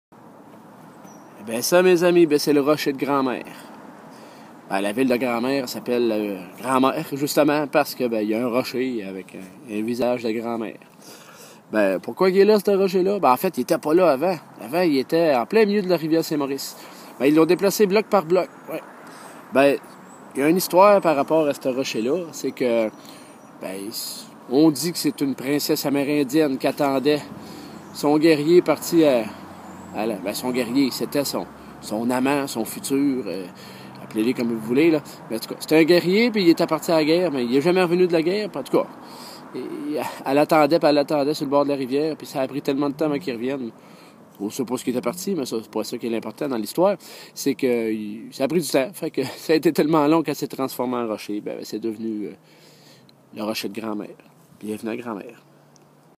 P'tite tranche de vie # 1
Je profite de mon escapade dans ma Mauricie natale pour vous raconter pourquoi il y a une ville qui s'appelle Grand-Mère. C'est à cause d'une grosse face de grand-mère dans une grosse roche.